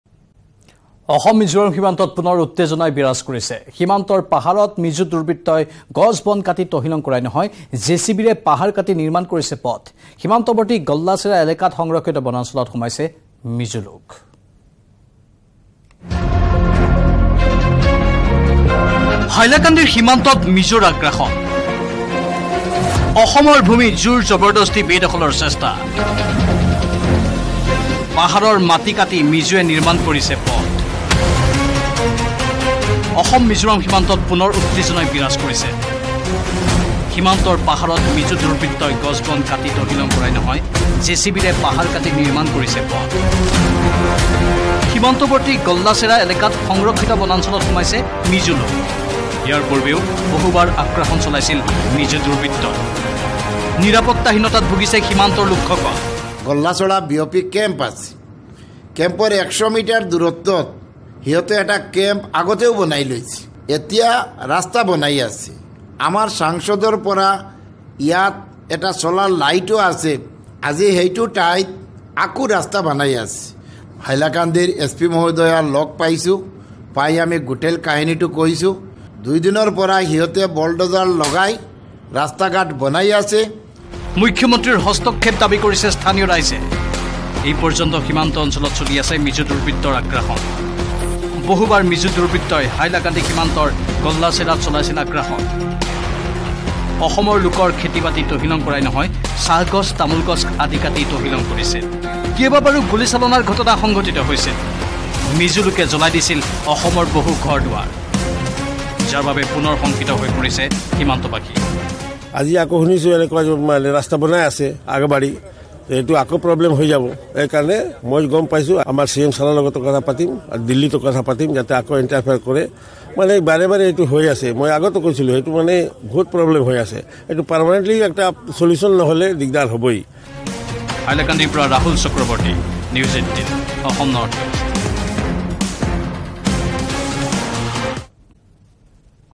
Assam-Mizoram Border Conflict | অসম-মিজোৰাম সীমান্তত পুনৰ উত্তেজনা বিৰাজ কৰিছে | Assamese News
Assam-Mizoram Border Conflict | অসম-মিজোৰাম সীমান্তত পুনৰ উত্তেজনা বিৰাজ কৰিছে | সীমান্তৰ পাহাৰত মিজো দুৰ্বৃত্তই গছ বন কাটি তহিলং কৰাই নহয়, জেচিবিৰে পাহাৰ কাটি নিৰ্মাণ কৰিছে পথ। সীমান্তৱৰ্তী গল্লাছেৰা এলেকাত সংৰক্ষিত বনাঞ্চলত সোমাইছে মিজো লোক।